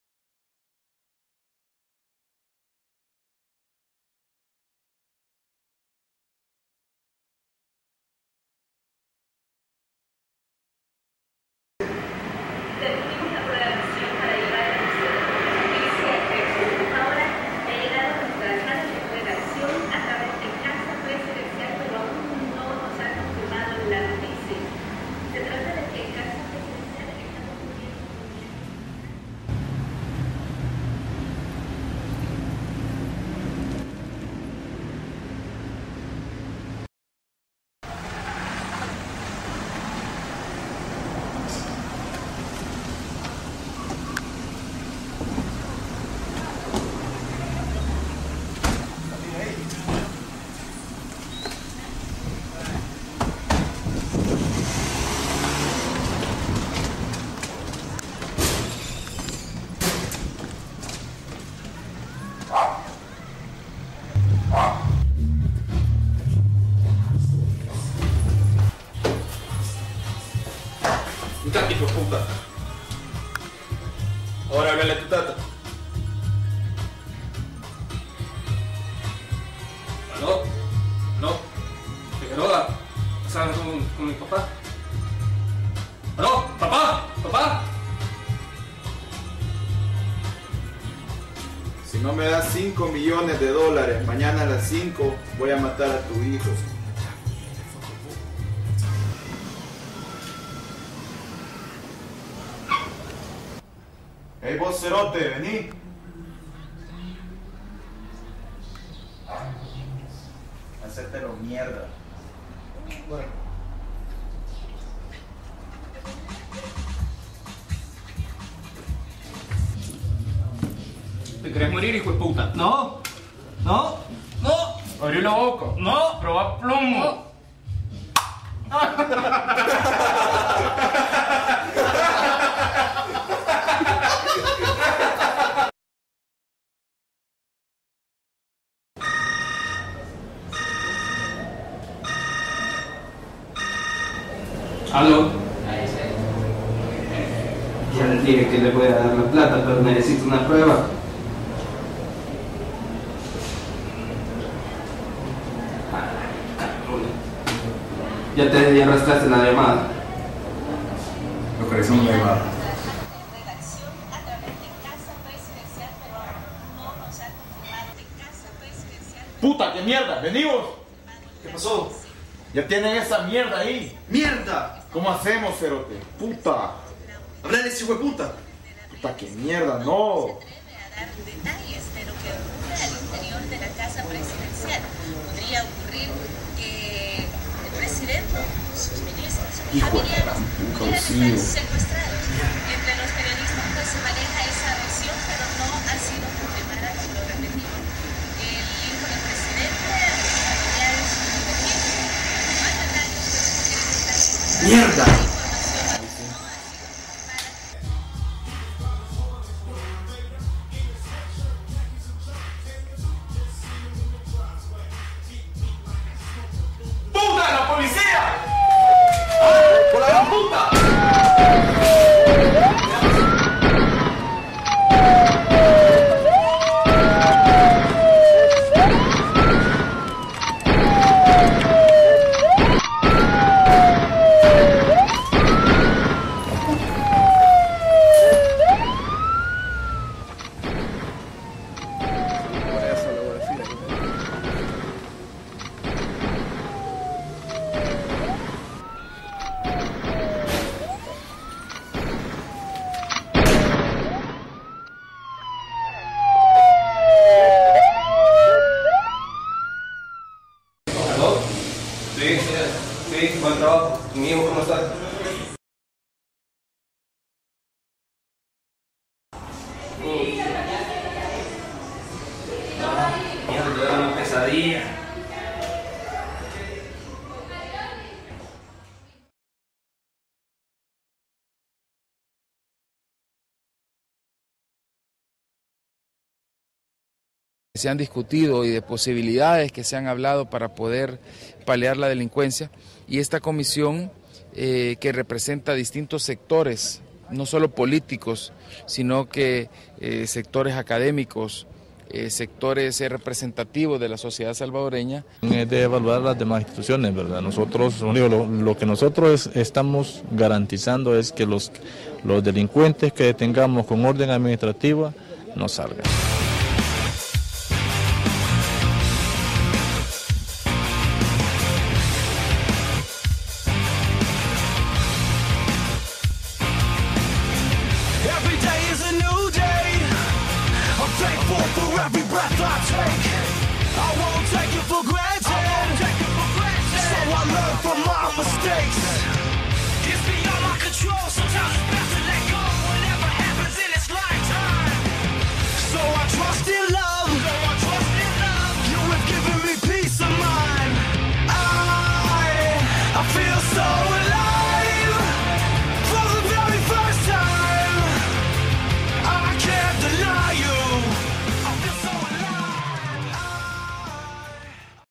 UNIVERSIDAD DE EL SALVADOR AUDIOVISUALES- DELINCUENCIA-2006
Trabajos audiovisuales de los estudiantes de PE... (more)
Added: January 06, 2008
Trabajos audiovisuales de los estudiantes de PERIODISMO de la UES, precentados por la Asociasion de Estudiantes de Periodismo A.E.P